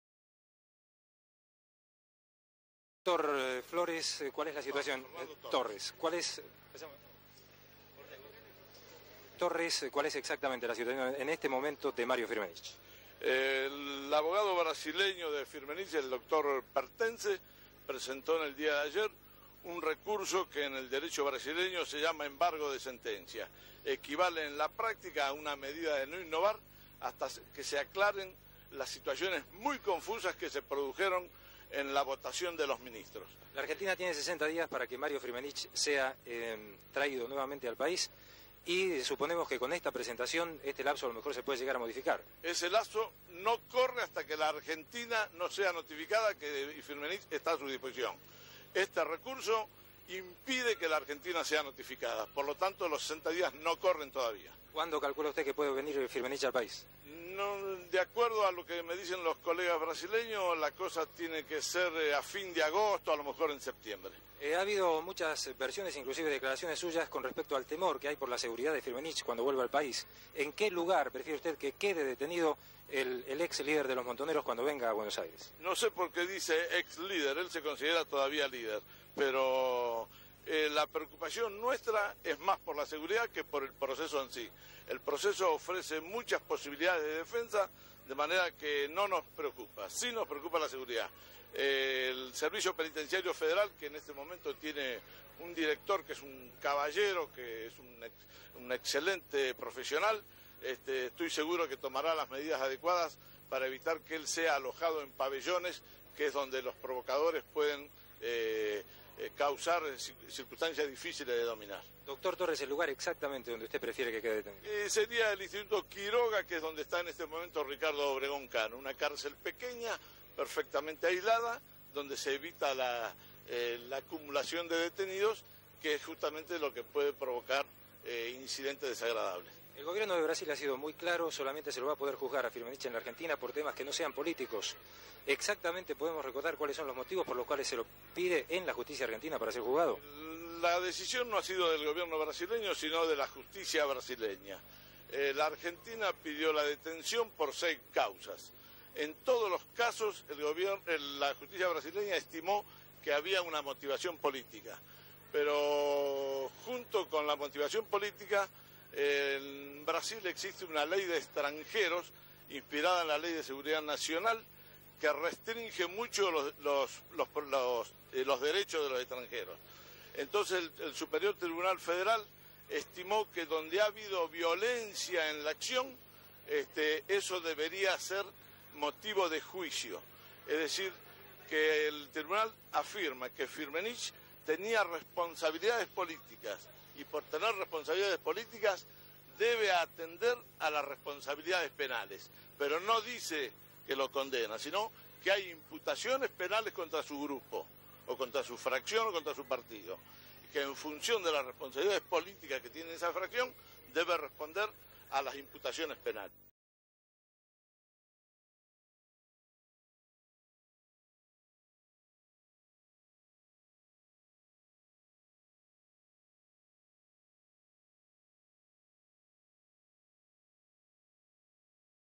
Eduardo Torres detencion de Mario Firmenich en Brasil 1984
Bs. As.: Reportaje al Dr. Eduardo Torres: -¿Cuál es exactamente la situación en este momento de Mario Firmenich? -¿La Argentina tiene 60 días para que Mario Firmenich sea traído nuevamente al país y suponemos que con esta presentación este lapso a lo mejor se puede llegar a modificar? -¿Cuándo calcula usted que puede venir Firmenich al país? -¿Ha habido muchas versiones inclusive declaraciones suyas con respeto al temor que hay por la seguridad de Firmenich cuando vuelva al país, en que lugar prefiere usted que quede detenido el ex líder de los montoneros cuando vuelva a Buenos Aires? -¿Dr. Torres, el lugar exactamente donde usted prefiere que quede detenido? -¿El gobierno de Brasil ha sido muy claro, solamente se lo va a poder juzgar a Firmenich en Argentina por temas que no sean políticos, exactamente podemos recordar cuales son los motivos por los cuales se lo piden en la justicia argentina para ser juzgado? (Franco Salomone)
Fecha: 18/6/1984
Duración: 3 minutos 23 segundos
Código: BG-0421